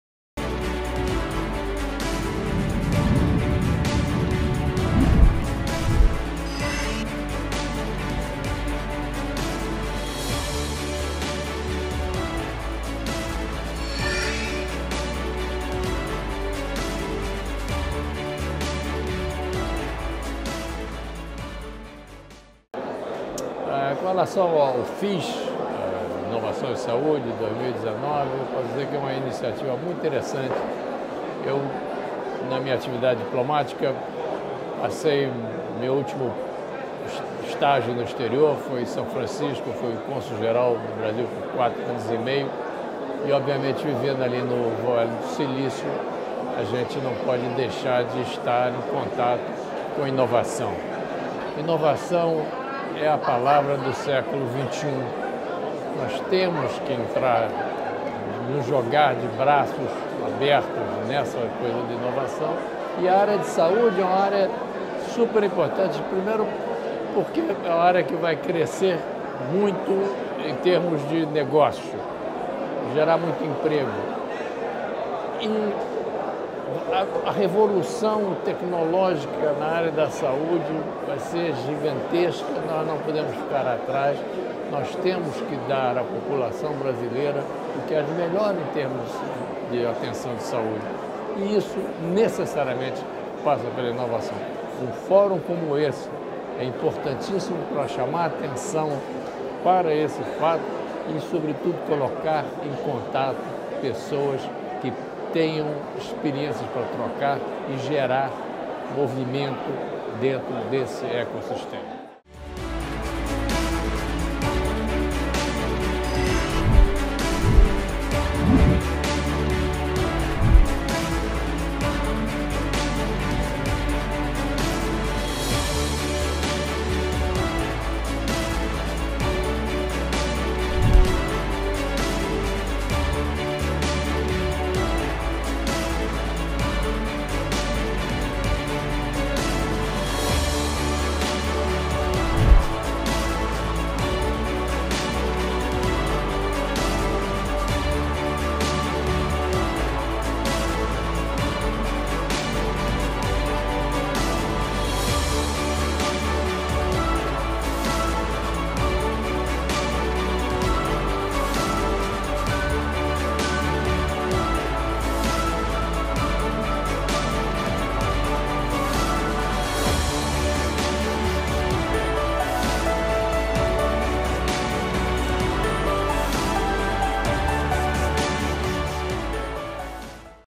Entrevista com Eduardo Prisco
O embaixador do Itamaraty Eduardo Prisco comentou sobre sua experiência como cônsul-geral nos Estados Unidos e disse que “vivendo próximo ao Vale do Silício, estive envolvido com inovação e percebi que a área de saúde vai crescer muito com a inovação, vai gerar muito emprego” e classificou o FIS19 como “importantíssimo para chamar atenção a isso”.